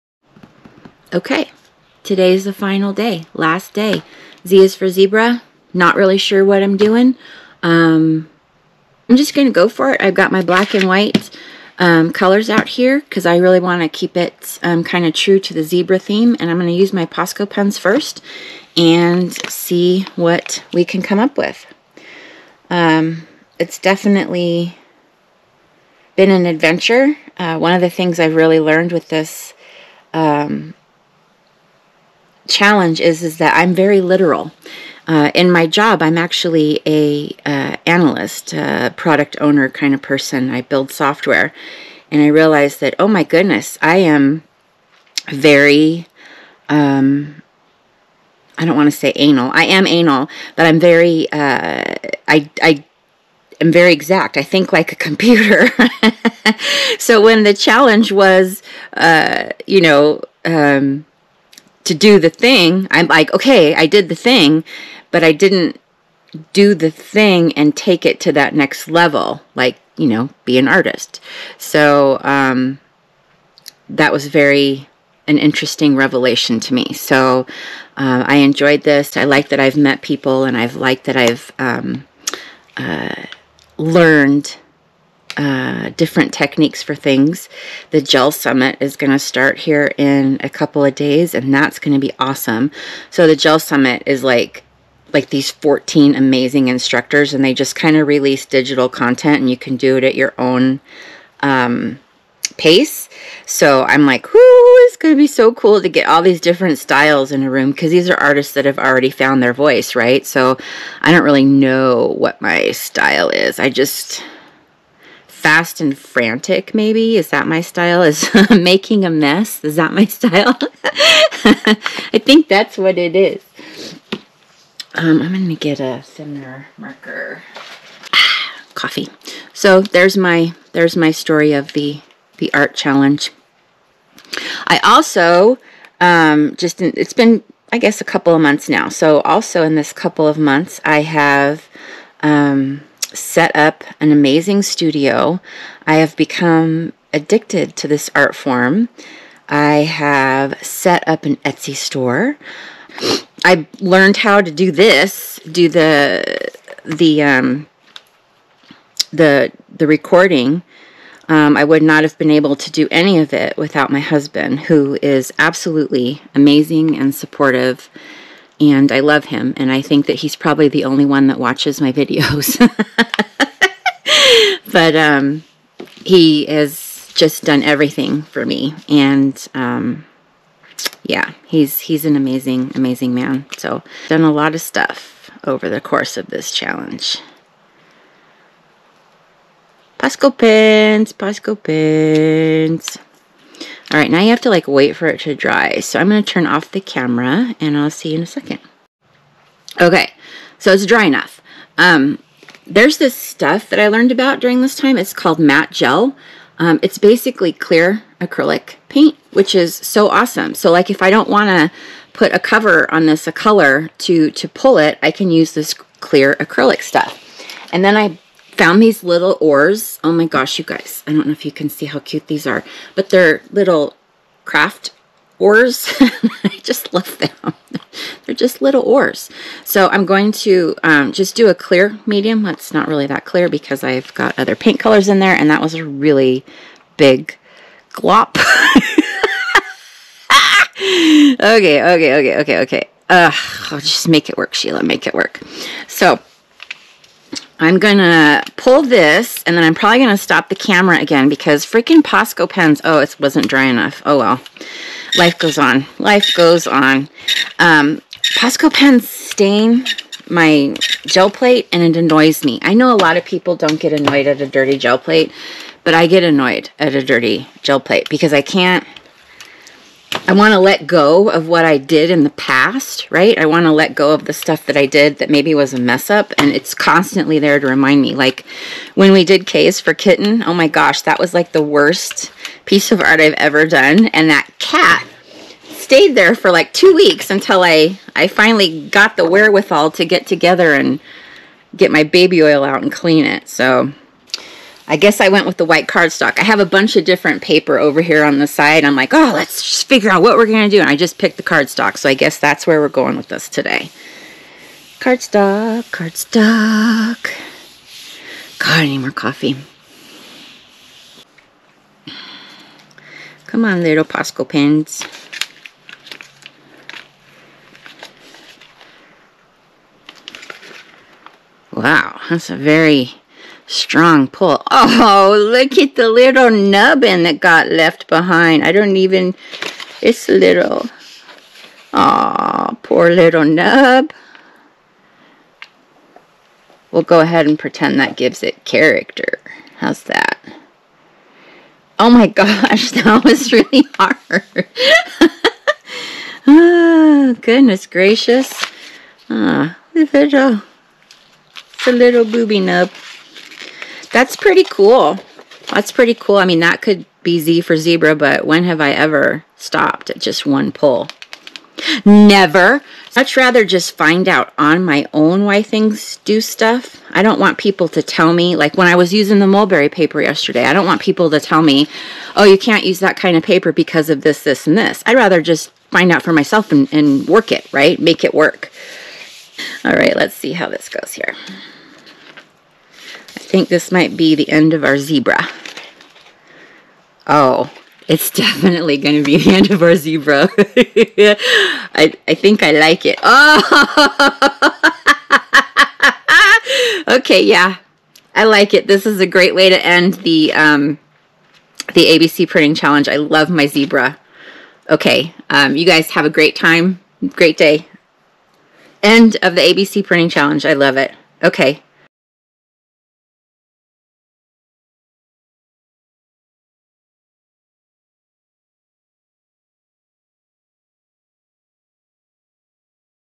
LAST DAY Z is for Zebra
Oh my gosh! It's here! The last day of the @gelliarts abcprintingchallenge! We are on "Z" for "Zebra" - and I'm going to miss this fun!

Today is "Talky Talk TUESDAY" - I just felt like it, so I did. Video is about 7 minutes and I full on realize I'm saying "Posca Pens" incorrectly. We are going to go with "a cute is THAT?!" instead of "Really? Can't you READ?"

I said this in the video - but I wanted to reiterate. I am so lucky to have the amazing husband that I have. He has done nothing but support me and help me get everything set up these last 2 months. He is the definition of unconditional love and so Doug (@salguod17), thank you. I love you. You're dead sexy. Thank you for all of it. Did you know?

Have a great day, go create something pretty and cause a little trouble along the way.